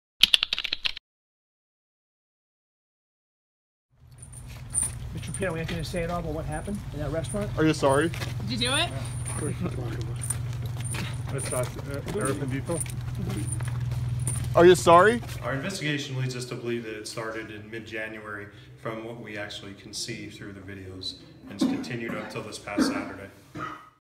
Preito Perp Walk
According to the complaint, Juan Prieto fled from Luigi’s restaurant, in Wright Township, after being spotting in the ceiling.

Wright Township Patrolman David  Winsock said the investigation led police to believe Prieto began taking photos in the ceiling of the pizzeria in mid-January and continued until Saturday when a woman noticed him. During his escape, police said, Prieto dropped his Nokia cellphone which was logged into evidence. Videos of female children under the age of 13 and female adults were allegedly found on the phone.